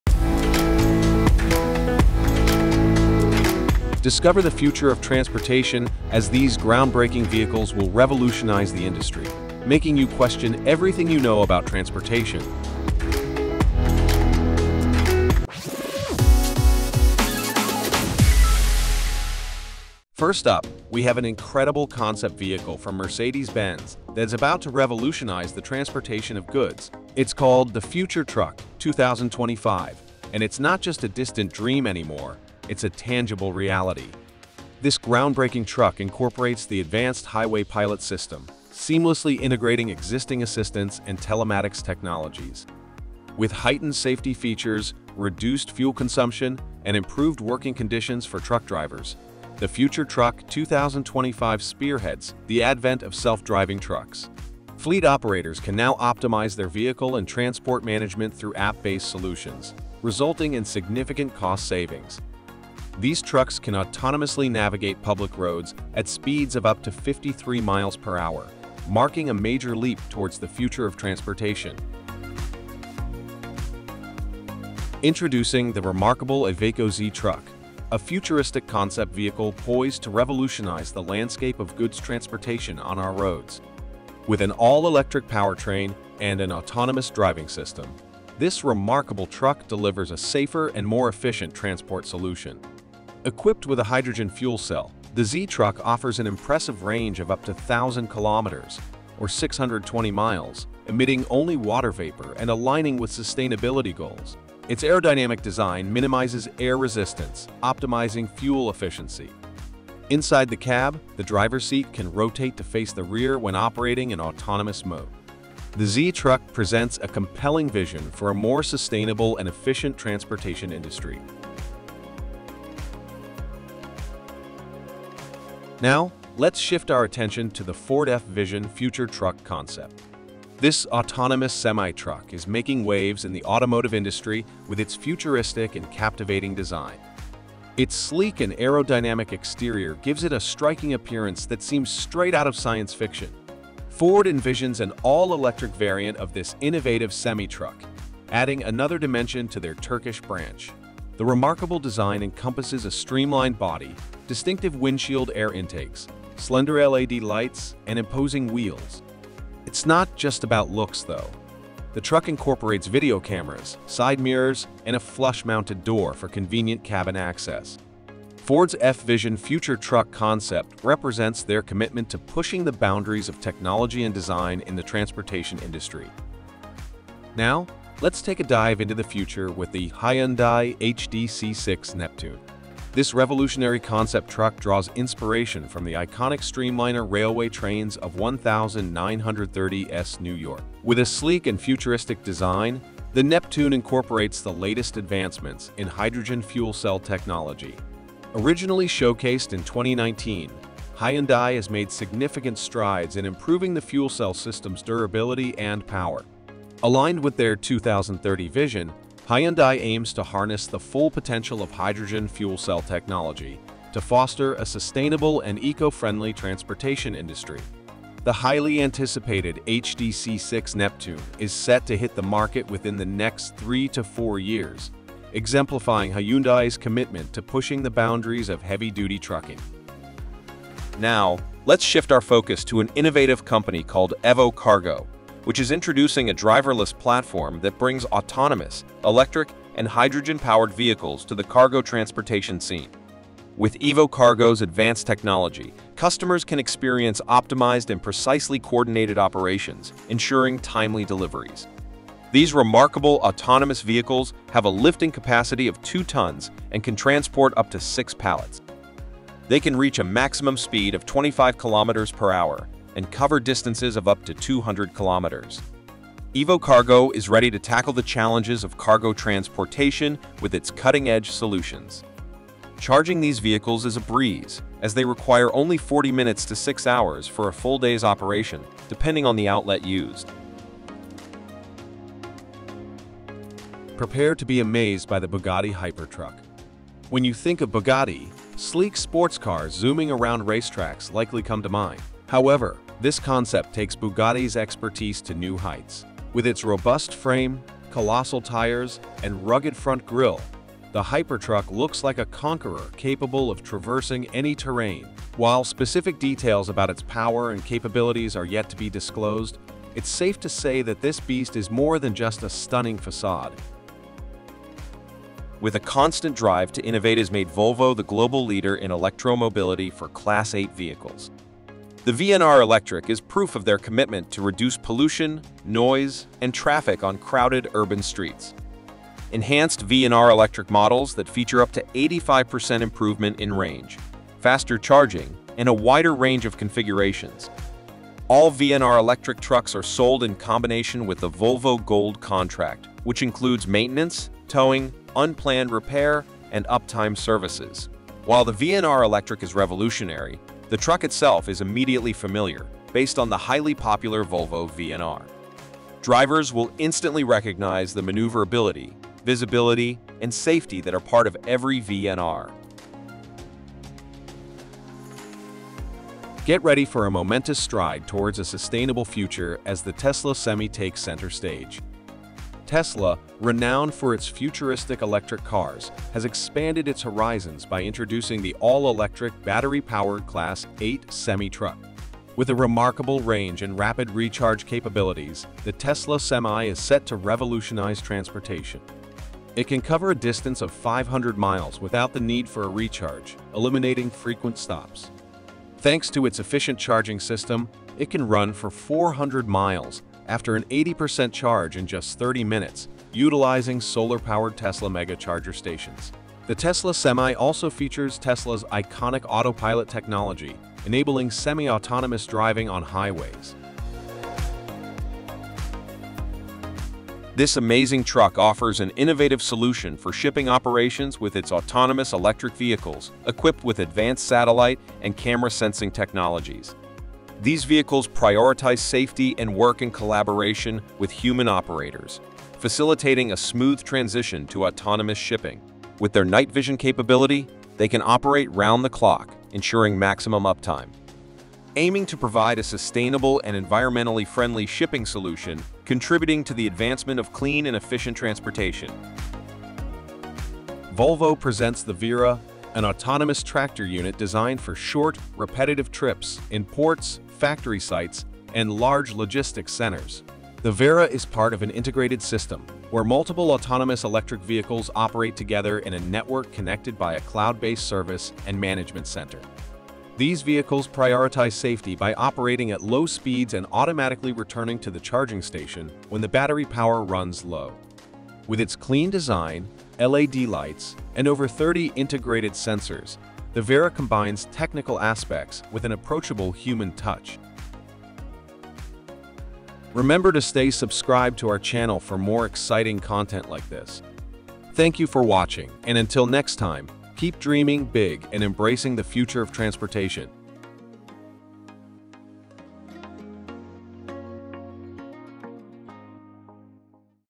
Top 10 Future Concept Trucks YOU MUST SEE
Discover a mesmerizing glimpse into the future of transportation with our video showcasing unseen futuristic trucks. Get ready to be amazed as we unveil groundbreaking designs and cutting-edge technology that are set to revolutionize the industry. Join us on this extraordinary journey as we explore the extraordinary trucks that have yet to be seen by many.